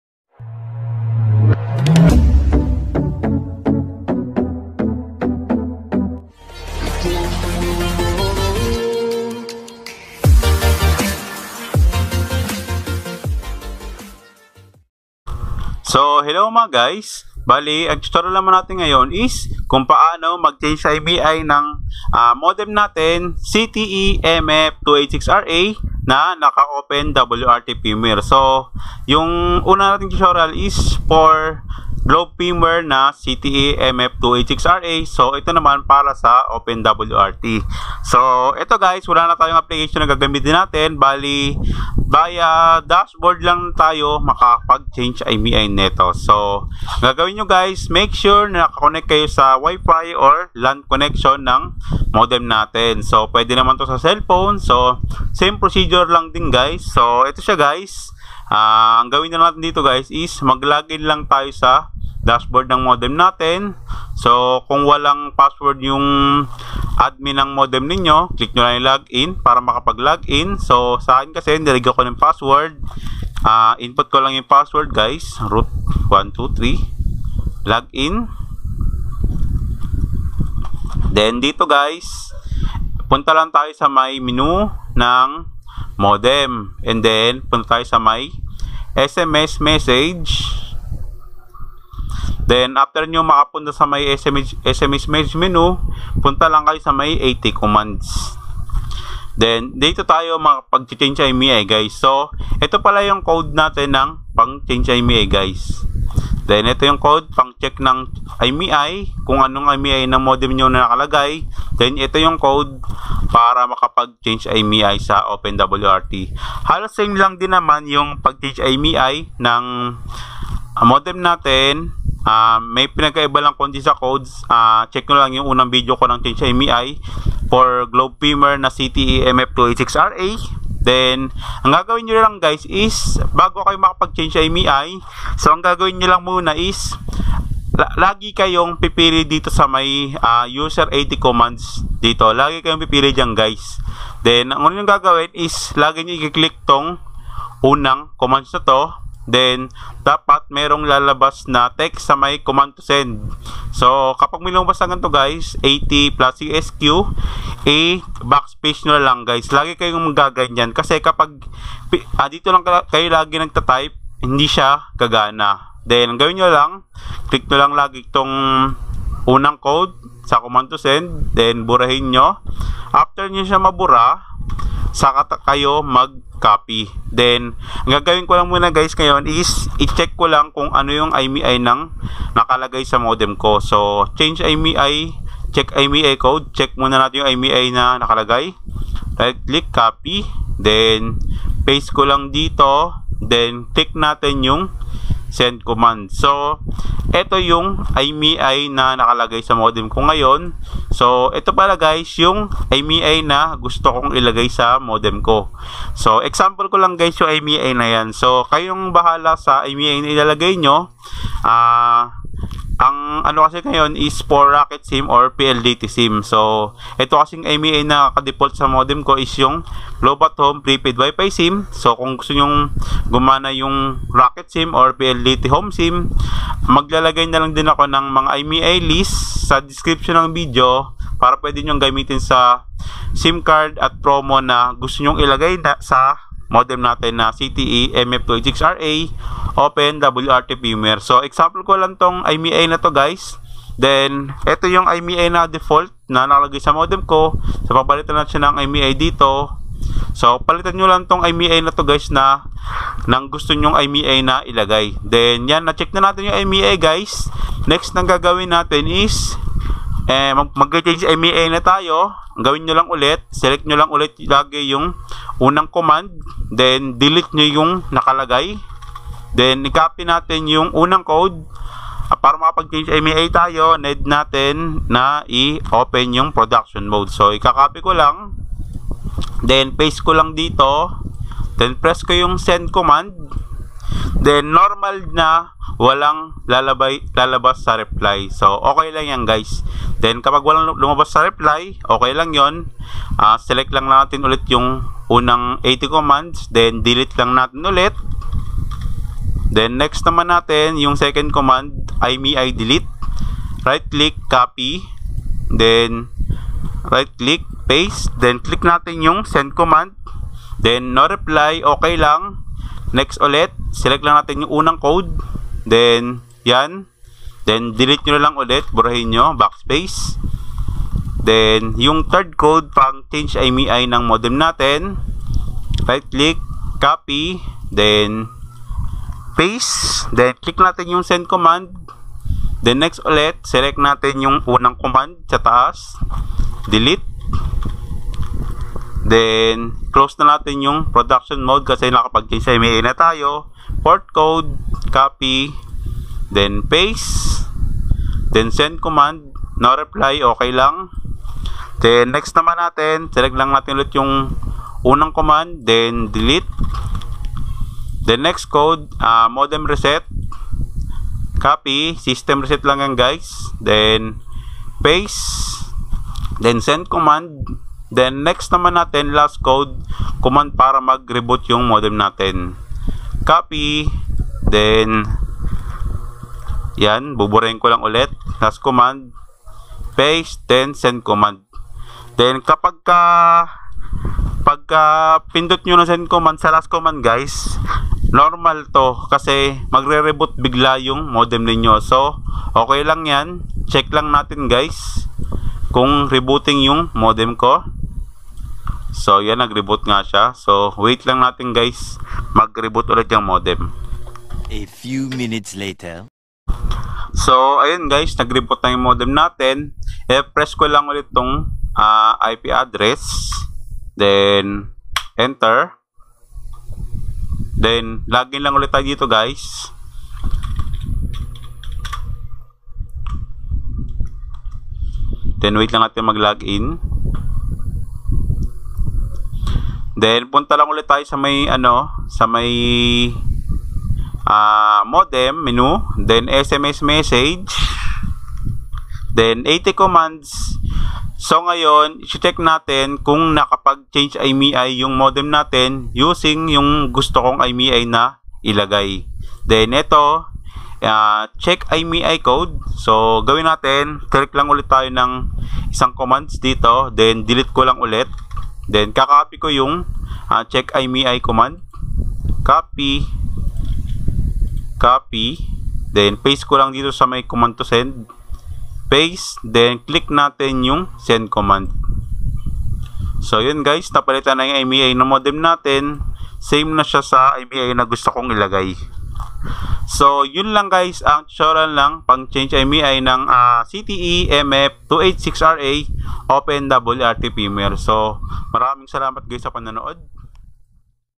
ZTE MF286RA OpenWRT Firmware Change IMEI Tutorial
- Smart Rocket Sim Prepaid IMEI
- PLDTAtHome - Smartbro Prepaid WiFi IMEI
- Globe At Home Prepaid Wifi IMEI

Thanks sa support